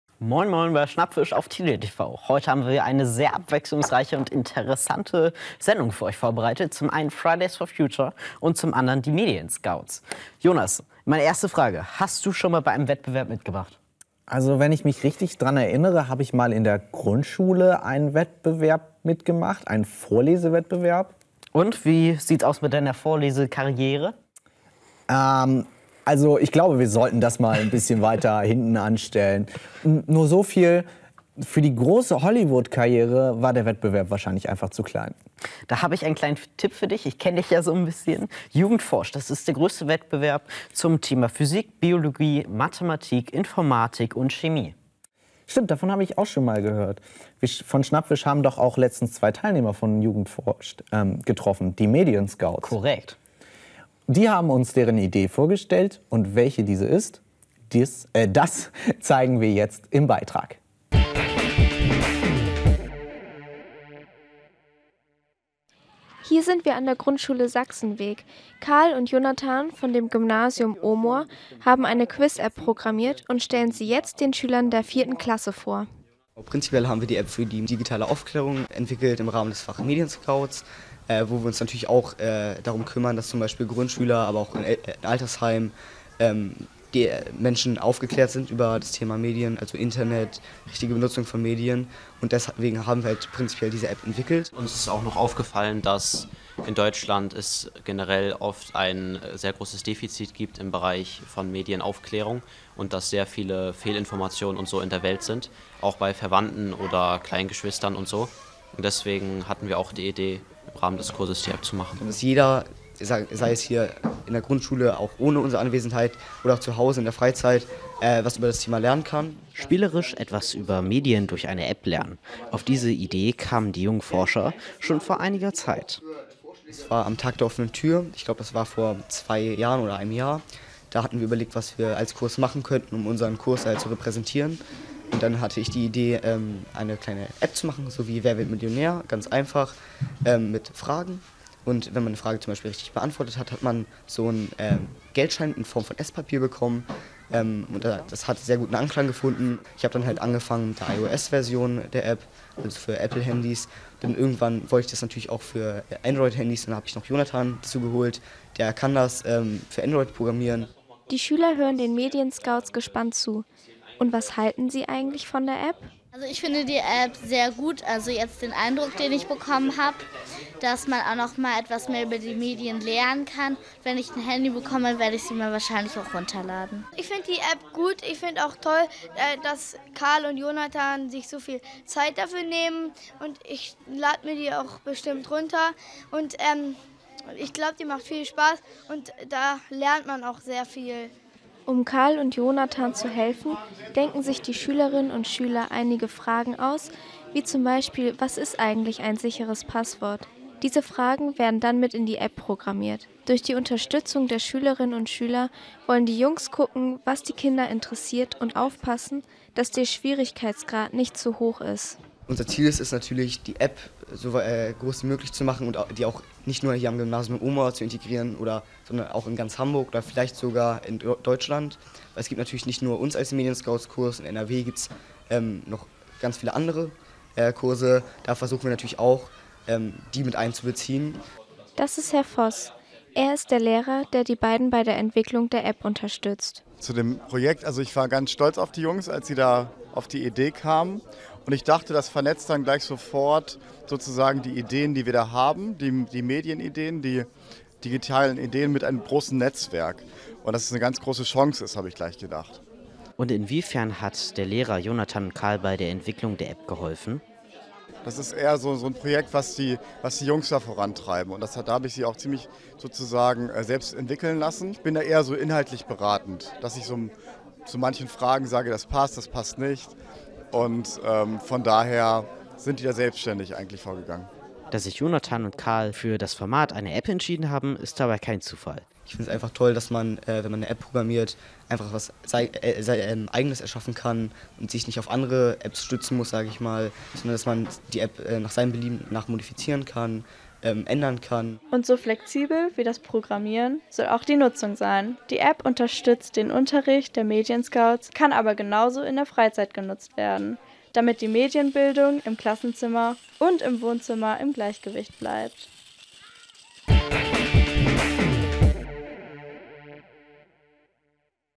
SchnappFisch - Unsere Zukunft beginnt jetzt! - MedienScouts#1: Die App
Eine App entwickeln? Damit für digitale Aufklärung sorgen? Klingt nach viel Arbeit, ist aber genau das Ding für die beiden Medienscouts Carl und Jonathan.

Wie war die Teilnahme am Jugend Forscht Regionalwettbewerb für Carl & Jonathan? Hier gehts zu Medienscouts #2